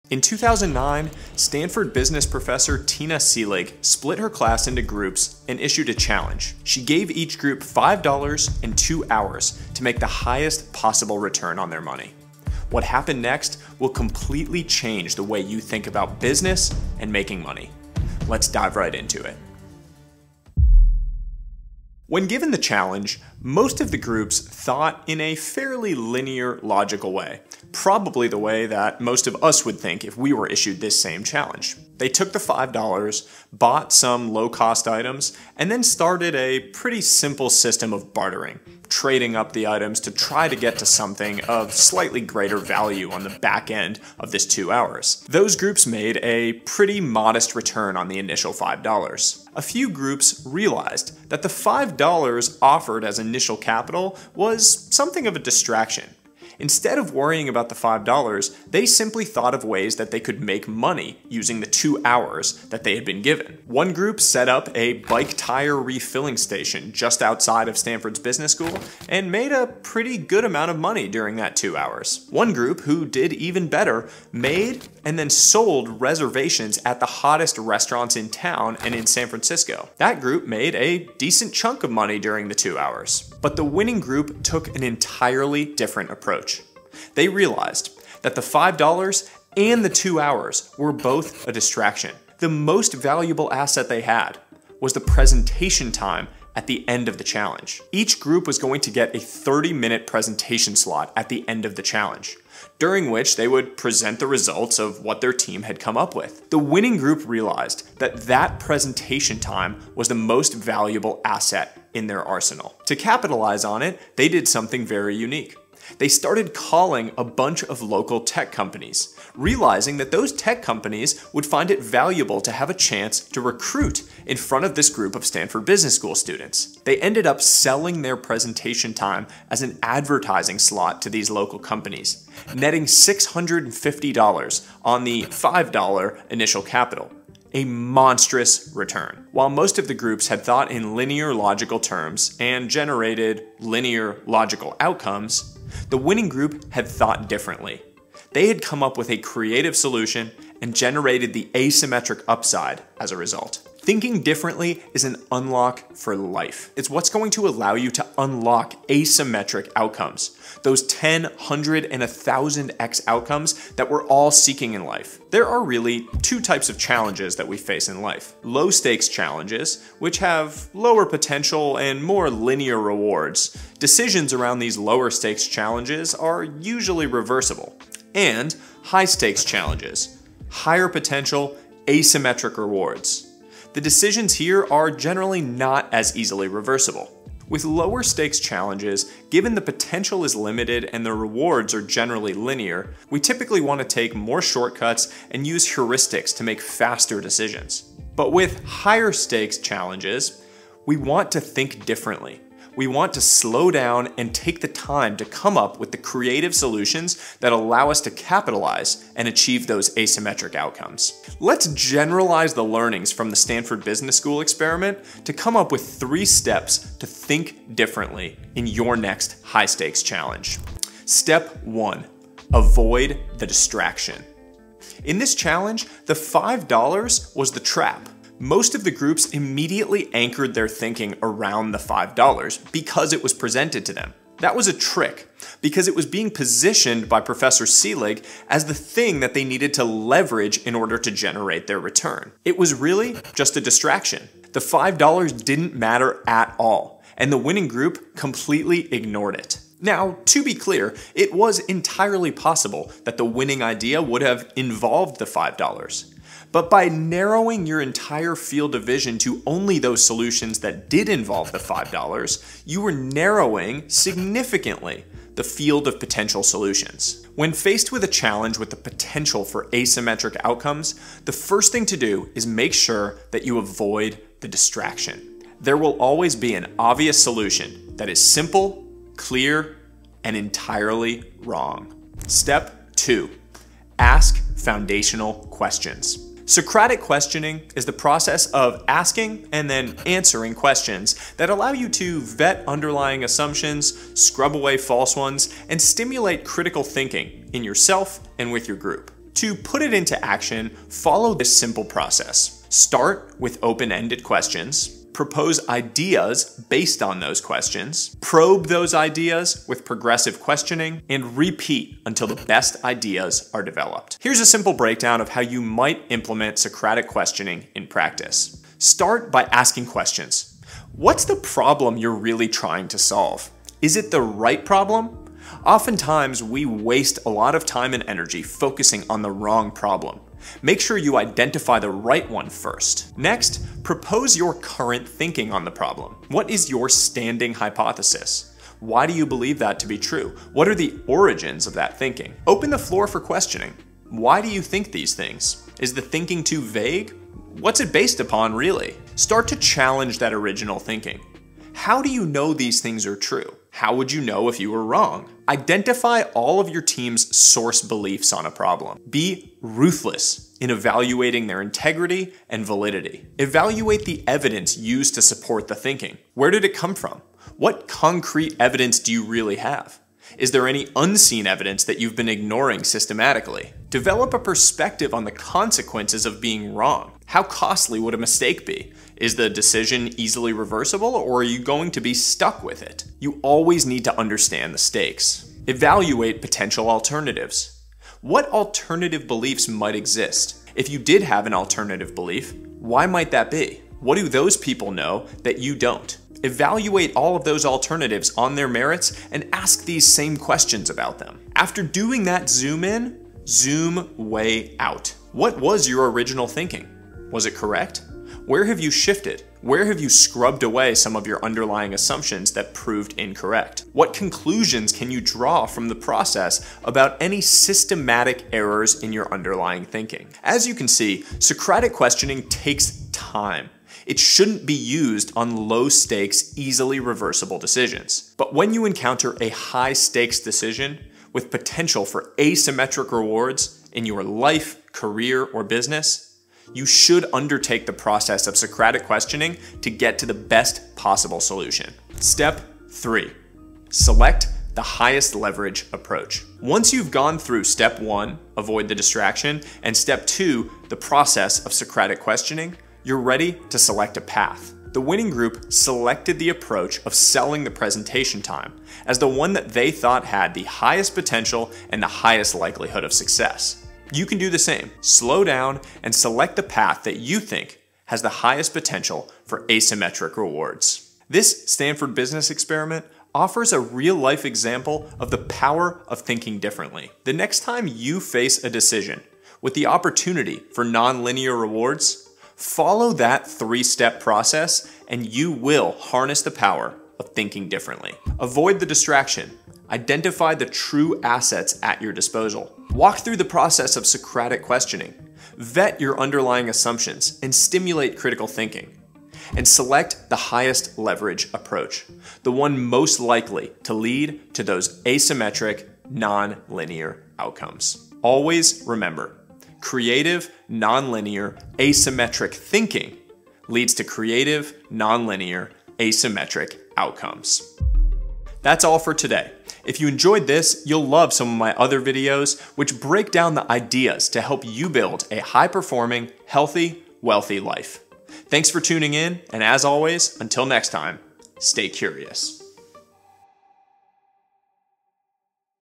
How To Turn $5 Into $650 in 2 Hours (Think Differently)
This Stanford Business School experiment will change the way you think about business and making money forever. It's a powerful example of the impact of thinking differently.

When you identify high-stakes opportunities and apply creative, non-linear thinking, you can expect to see uncommon outcomes.

In this video, I'll share the three critical steps to thinking differently, plus the common pitfalls holding you back from achieving those uncommon, asymmetric results.

00:00 - This Stanford Experiment Will Change How You Think About Business & Making Money
00:22 - Most Groups Made This Common Mistake (Avoid It)
00:49 - Some Groups Thought Slightly Differently (Avoid It)
01:23 - The Winning Group's Unique Approach
02:48 - Two Types of Challenges in Life (Know the Difference)
03:11 - When to Think Differently (It's Not Always Necessary)
03:35 - 3 Steps to Think Differently in High-Stakes Opportunities
03:44 - Step 1: Avoid Distractions Like a Pro
04:45 - Step 2: Ask the Right Foundational Questions
05:06 - Socratic Questioning 101
07:25 - Step 3: Choose the Highest Leveraged Approach
08:12 - Make High-Leverage Decisions for Asymmetric Outcomes